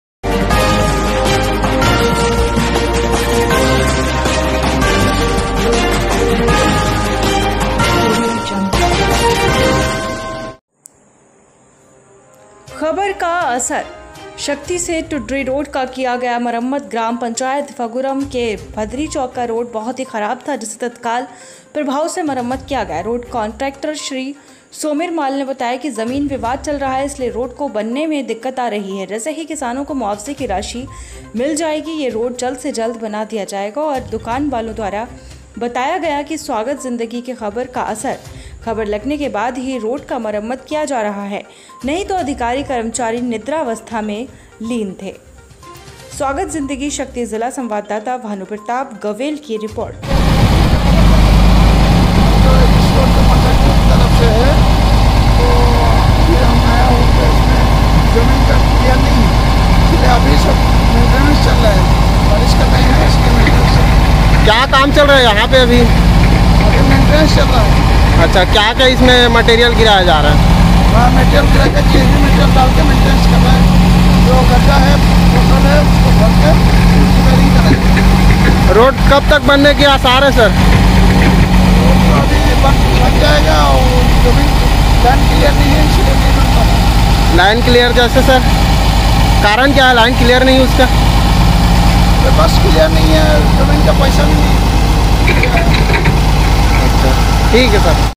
स्वागत जिंदगी न्यूज़ की खबर का असर देखने को मिला, बहुत जल्द शक्ति से टुड्री रोड का किया गया मरम्मत..
*खबर का असर* संवाददाता भानु प्रताप गवेल की रिपोर्ट

शक्ति से टुड्री रोड का किया गया मरम्मत ग्राम पंचायत फगुरम के भदरी चौक का रोड बहुत ही खराब था जिसे तत्काल प्रभाव से मरम्मत किया गया रोड कान्ट्रेक्टर श्री सोमिर माल ने बताया की जमीन विवाद चल रहा है इसलिए रोड को बनने में दिक्कत आ रही है जैसे ही किसानों को मुआवजे की राशि मिल जाएगी यह रोड जल्द से जल्द बना दिया जाएगा और दुकान वालों द्वारा बताया गया कि स्वागत जिंदगी के खबर का असर. खबर लगने के बाद ही रोड का मरम्मत किया जा रहा है नहीं तो अधिकारी कर्मचारी निद्रा अवस्था में लीन थे

स्वागत जिंदगी शक्ति जिला  संवाददाता भानु प्रताप गवेल की रिपोर्ट